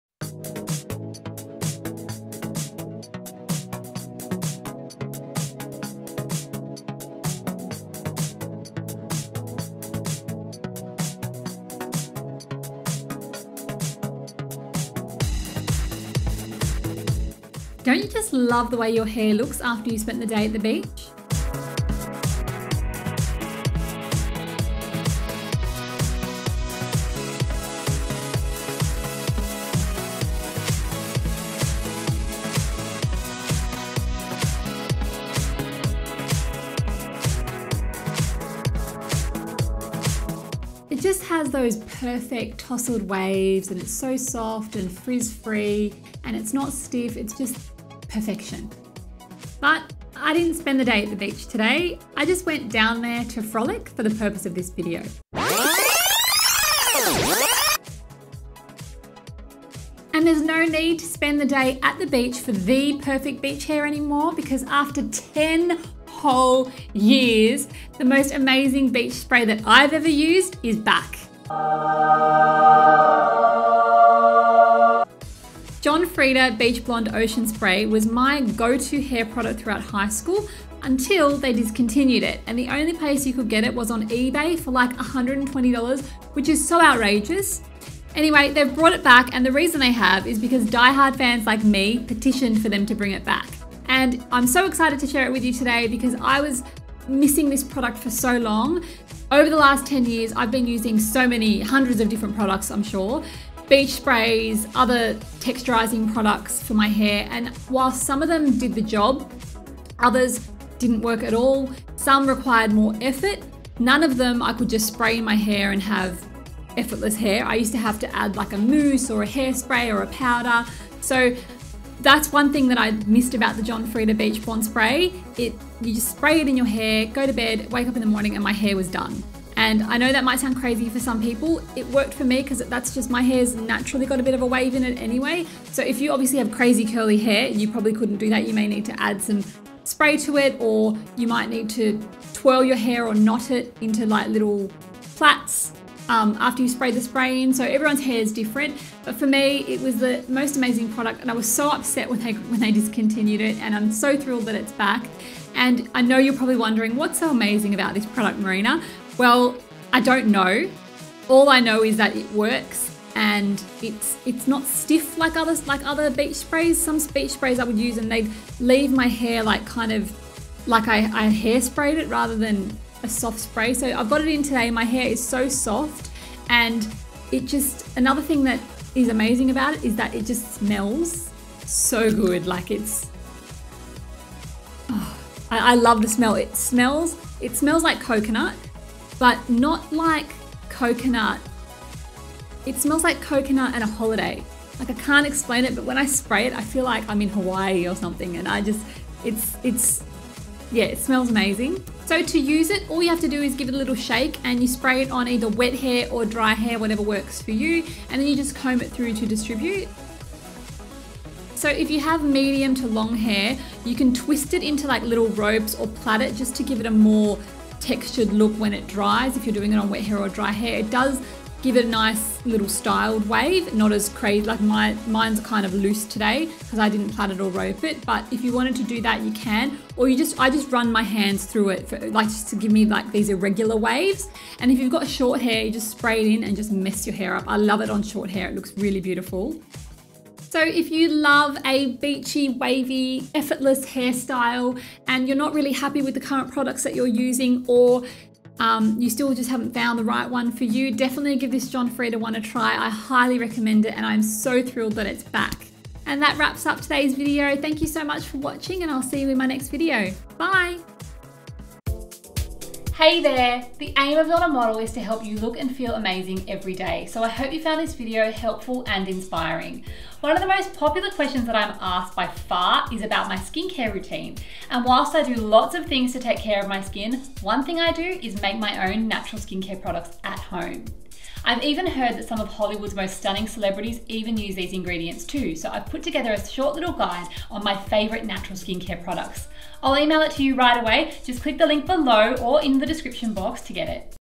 Its back! John Frieda Beach Blonde Ocean Waves Sea Salt Hair Spray | Review
John Frieda Beach Blonde Ocean Waves Sea Salt Hair Spray
⬇️⬇️⬇️❤️OPEN ME❤️⬇️⬇️⬇️

Hello my fashionsistas!

I am SO excited to publish my first video for 2015 because it’s about one of my all time favorite hair products.

Have you ever had a favorite product get discontinued? Gone. Nada. Buh-Byeee!

Well TEN years ago John Frieda did just that, they discontinued their most amazing product, Beach Blonde Ocean Waves Sea Salt Hair Spray. Well maybe it’s their second most amazing product – have you tried their De Frizz Serum? It’s the bomb!

Back when I was in high school (yes 10 years ago – eek!) I used the Beach Blonde Ocean Waves Sea Salt Hair Spray every day, it was the easiest way for me to do my hair, I would actually spray it into my wet hair the night before and wake up with perfectly tousled hair. Basically this product worked perfectly for me, and I was just gutted when it got discontinued.

So if you love the most perfect, effortless and sexy beach hair or have ever had a product get discontinued before then you know exactly how I feel right now!

And remember, you don’t need to be a model to look like you’ve stepped off the runway – you just need a killer wardrobe and some lipstick!

Big love,
xox Marina

The lovely team at John Frieda asked me if I would be interested in trying out a cult product that had been discontinued and I almost fell off my chair when I got the email - because I knew the rumors of the best sea salt spray coming back into the world must be true!